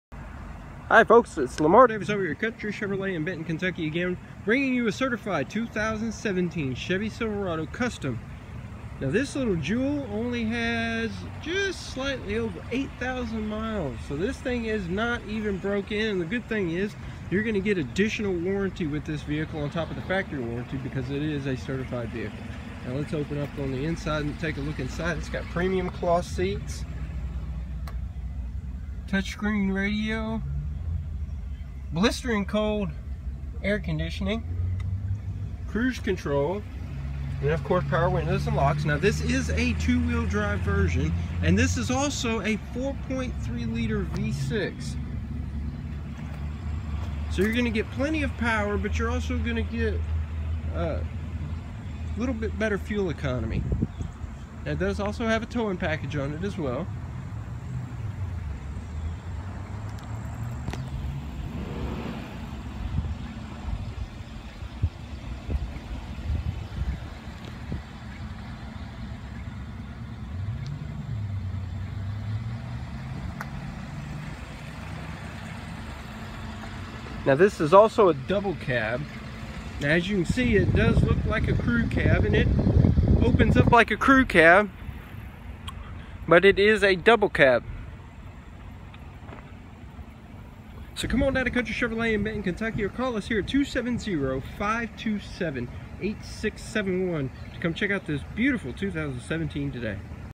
Certified 2017 Chevrolet Silverado custom
Certified 2017 Chevrolet Silverado custom double cab with 8k miles video made by Lamar Davis Internet Manager at Country Chevrolet in Benton, Ky. For more information or to getting pricing, payments and availability call and ASK FOR Lamar Davis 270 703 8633 or visit our